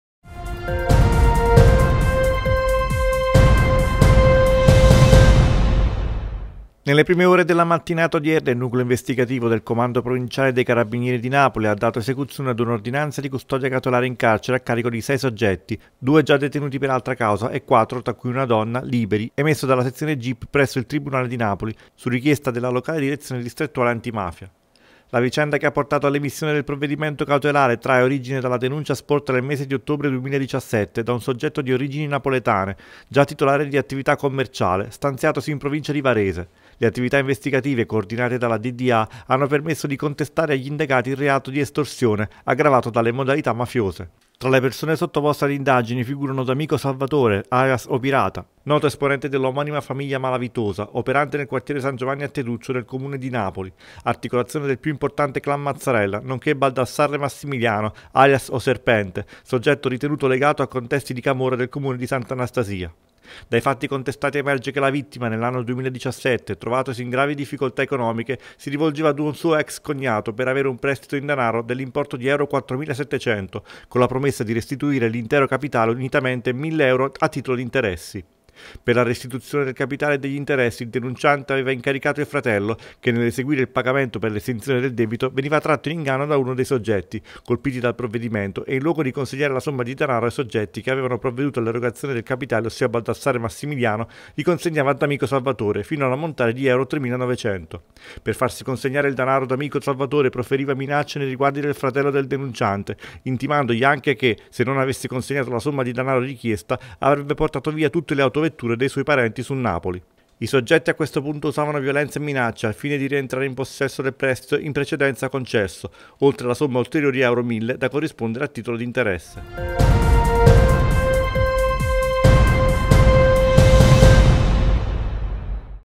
Camorra, 6 persone del Clan Mazzarella in manette per estorsione
Nelle prime ore della mattinata odierna il Nucleo Investigativo del Comando Provinciale dei Carabinieri di Napoli ha dato esecuzione ad un'ordinanza di custodia cautelare in carcere a carico di sei soggetti - due già detenuti per altra causa e quattro, tra cui una donna, liberi - emesso dalla Sezione G.I.P. presso il Tribunale di Napoli su richiesta della locale Direzione Distrettuale Antimafia.
La vicenda che ha portato all’emissione del provvedimento cautelare trae origine dalla denuncia sporta nel mese di ottobre 2017 da un soggetto di origini napoletane, già titolare di attività commerciale, stanziatosi in provincia di Varese. Le attività investigative, coordinate dalla DDA, hanno permesso di contestare agli indagati il reato di estorsione, aggravato dalle modalità mafiose.
Tra le persone sottoposte ad indagini figurano D’AMICO Salvatore, alias O’ Pirata, noto esponente dell’omonima famiglia malavitosa, operante nel quartiere S. Giovanni a Teduccio del Comune di Napoli, articolazione del più importante clan Mazzarella, nonché BALDASSARRE Massimiliano, alias O’ Serpente, soggetto ritenuto legato a contesti di camorra del Comune di S. Anastasia.
Dai fatti contestati emerge che la vittima, nell’anno 2017, trovatosi in gravi difficoltà economiche, si rivolgeva ad un suo ex cognato, per avere un prestito in denaro dell’importo di euro 4.700 con la promessa di restituire l'intero capitale unitamente 1.000 euro a titolo di interessi.
Per la restituzione del capitale e degli interessi, il denunciante aveva incaricato il fratello che, nell’eseguire il pagamento per l’estinzione del debito, veniva tratto in inganno da uno dei soggetti, colpiti dal provvedimento, e in luogo di consegnare la somma di danaro ai soggetti che avevano provveduto all’erogazione del capitale ossia BALDASSARRE Massimiliano, li consegnava a D’AMICO Salvatore, fino all’ammontare di euro 3.900. Per farsi consegnare il danaro D’AMICO Salvatore proferiva minacce nei riguardi del fratello del denunciante intimandogli anche che, se non avesse consegnato la somma di danaro richiesta, avrebbe portato via tutte le autovetture dei suoi parenti su Napoli.
I soggetti, a questo punto, usavano violenza e minacce al fine di rientrare in possesso del prestito in precedenza concesso, oltre la somma di ulteriori euro 1.000 da corrispondere a titolo di interesse.